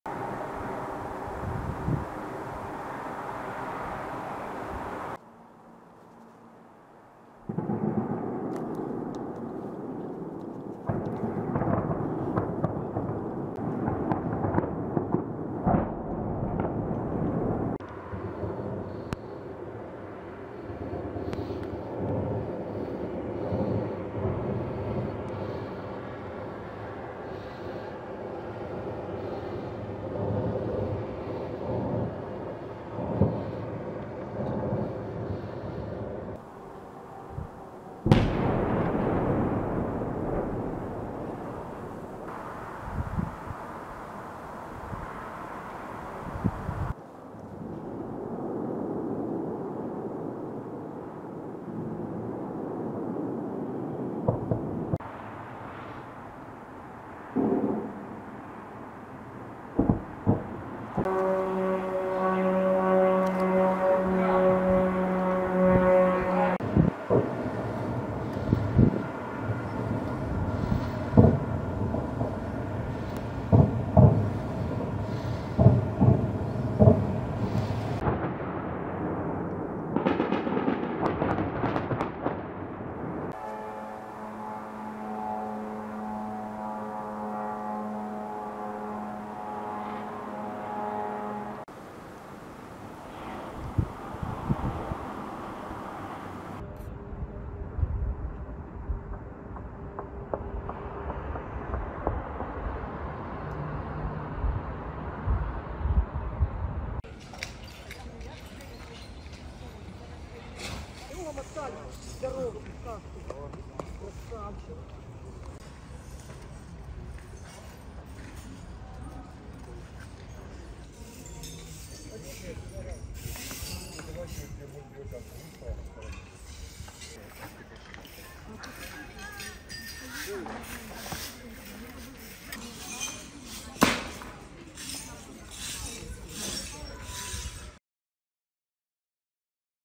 Ukraine's capital HIT by Russia's largest drone attack: 74 drones downed in assault on Kyiv
Ukraine's capital suffered what officials said was Russia's largest drone attack of the war on Saturday (November 25), leaving five people wounded as the rumble of air defences and explosions woke residents at sunrise.

The attack began hitting different districts of Kyiv in the early hours of Saturday, with more waves coming as the sun came up. The air raid warning lasted six hours.

Air force chief Mykola Oleschuk said 74 of the 75 drones launched at Ukraine had been downed.